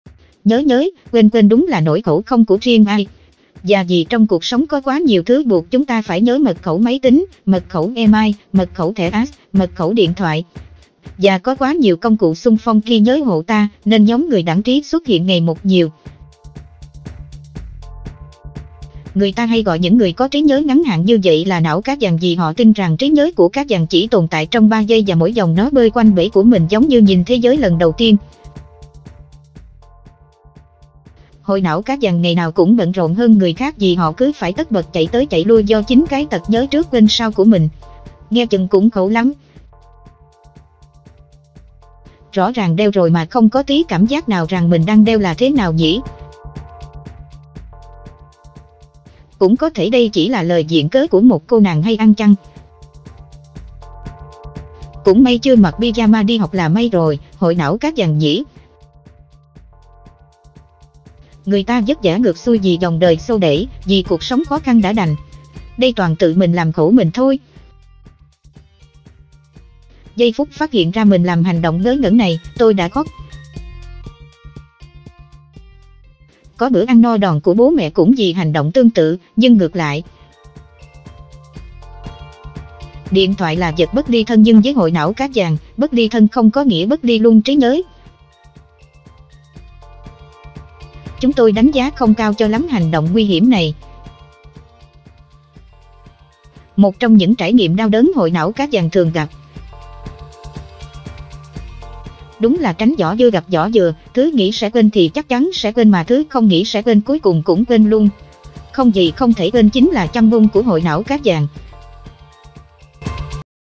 ✅ Hội 'não cá vàng' đau khổ: Dành cả thanh xuân để tìm kính, chìa khóa, điện thoại... dù đồ đang nga
Đặt Mua  Qua ZALO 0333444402

✅Nhớ nhớ, quên quên đúng là nỗi khổ không của riêng ai. Và vì trong cuộc sống có quá nhiều thứ buộc chúng ta phải nhớ (mật khẩu máy tính, mật khẩu email, mật khẩu thẻ ATM, mật khẩu điện thoại. ) và có quá nhiều công cụ xung phong ghi nhớ hộ ta, nên nhóm người đãng trí xuất hiện ngày một nhiều.

Người ta hay gọi những người có trí nhớ ngắn hạn như vậy là "não cá vàng" vì họ tin rằng trí nhớ của cá vàng chỉ tồn tại trong 3 giây và mỗi vòng nó bơi quanh bể của mình giống như nhìn thế giới lần đầu tiên.
Hội "não cá vàng" ngày nào cũng bận rộn hơn người khác vì họ cứ phải tất bật chạy tới chạy lui do chính cái tật nhớ trước quên sau của mình. Nghe chừng cũng khổ lắm!
Rõ ràng đeo rồi mà không có tí cảm giác nào rằng mình đang đeo là thế nào nhỉ?
Cũng có thể đây chỉ là lời viện cớ của một cô nàng hay ăn chăng?
Cũng may chưa mặc pijama đi học là may rồi, hội não cá vàng nhỉ?
Người ta vất vả ngược xuôi vì dòng đời xô đẩy, vì cuộc sống khó khăn đã đành. Đây toàn tự mình làm khổ mình thôi
Giây phút phát hiện ra mình làm hành động ngớ ngẩn này, tôi đã khóc...
Có bữa ăn no đòn của bố mẹ cũng vì hành động tương tự, nhưng ngược lại
Điện thoại là vật bất ly thân nhưng với hội não cá vàng, bất ly thân không có nghĩa "bất ly" luôn trí nhớ
Chúng tôi đánh giá không cao cho lắm hành động nguy hiểm này...
Một trong những trải nghiệm đau đớn hội não cá vàng thường gặp
Đúng là "tránh vỏ dưa gặp vỏ dừa", thứ nghĩ sẽ quên thì chắc chắn sẽ quên mà thứ không nghĩ sẽ quên cuối cùng cũng quên luôn. "Không gì không thể quên" chính là châm ngôn của hội "não cá vàng"